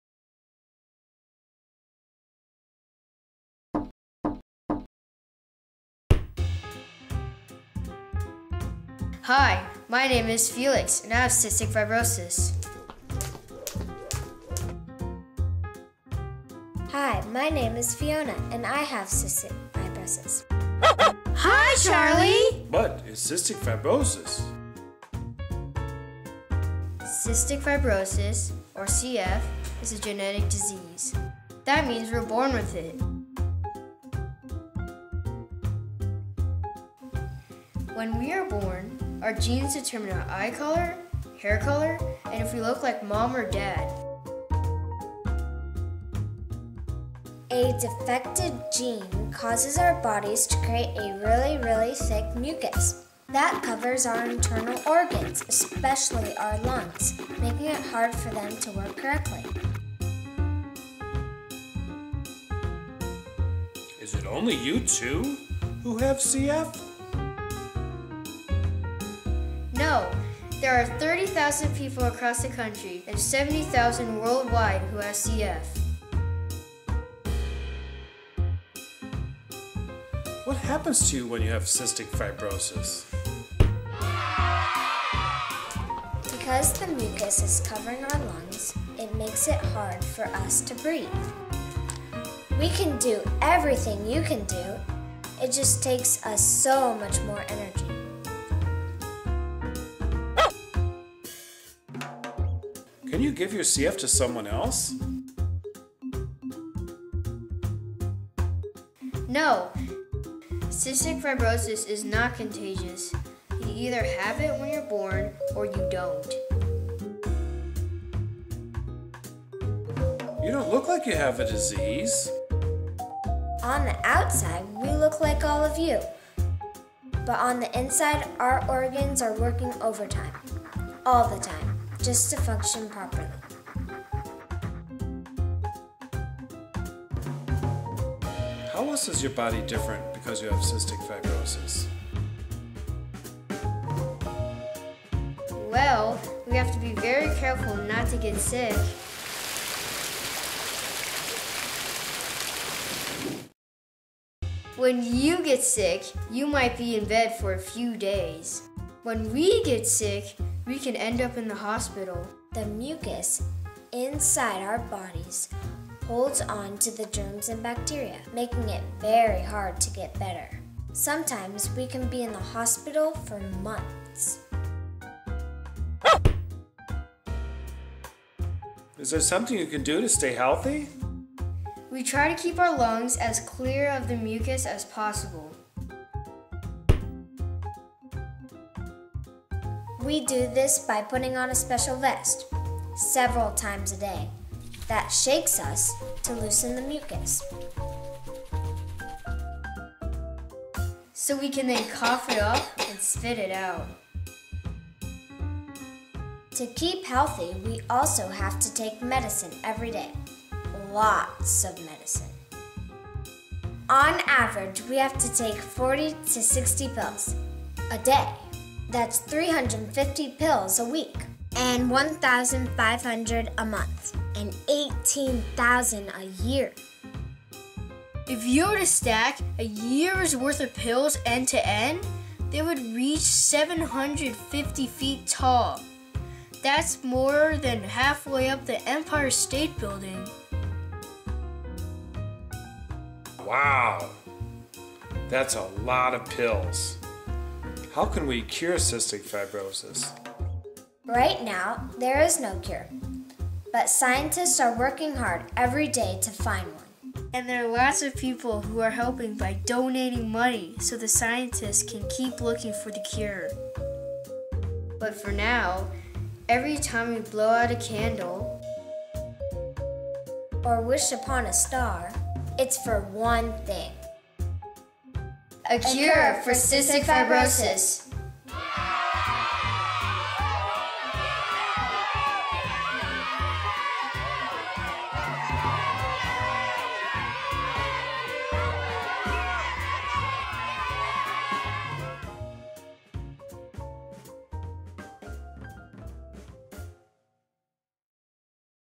What is cystic fibrosis? Animation
Animated  video provide facts about cystic fibrosis.  Created for San Diego Chapter of the Cystic Fibrosis Foundation's CureFinders in-school program.